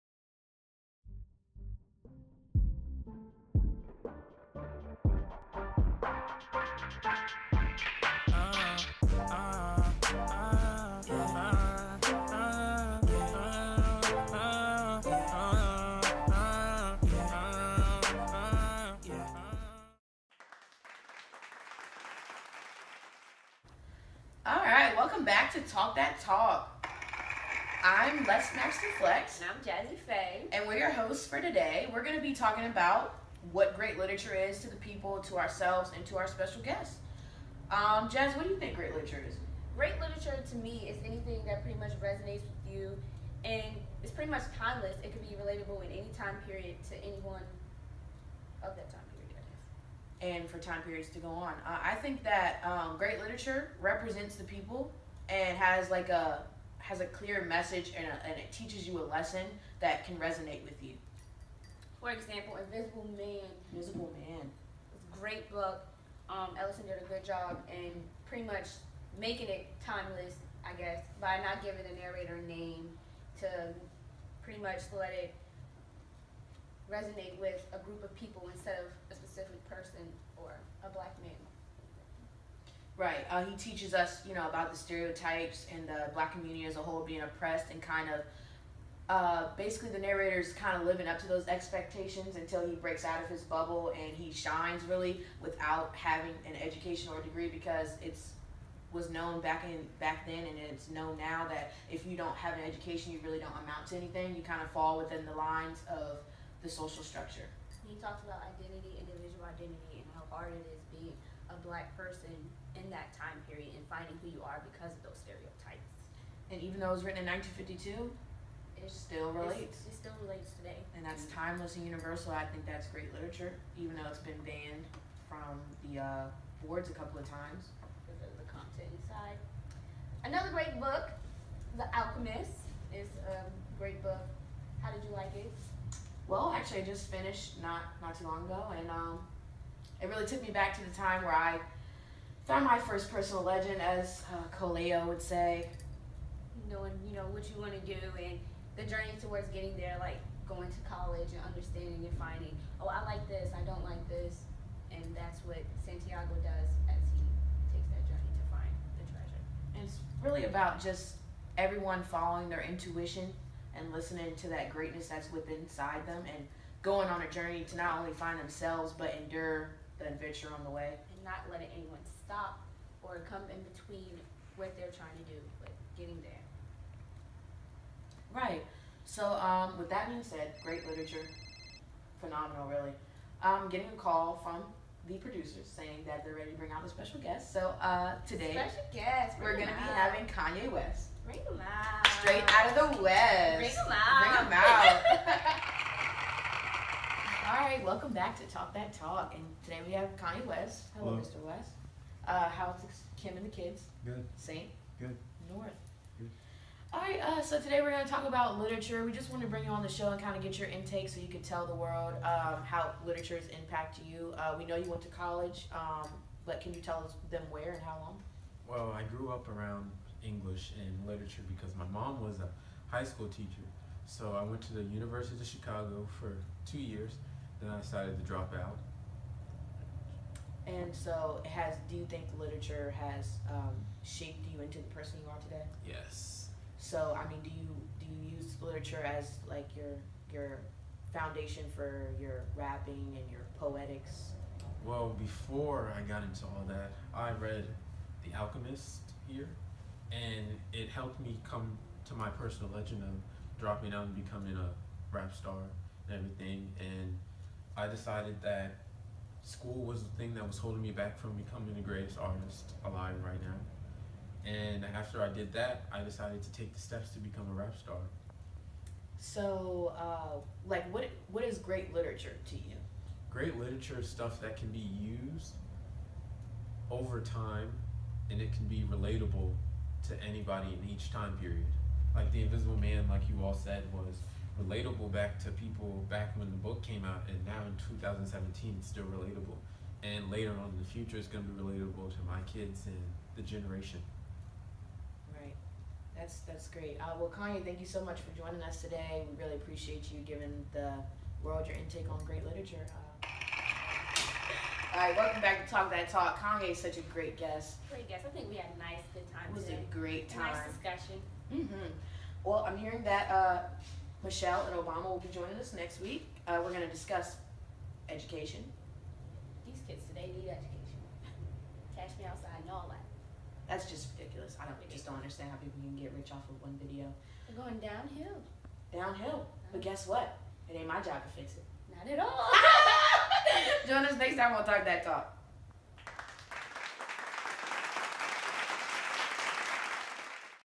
What Is Great Literature?
An English Final done by Bruton High School Students.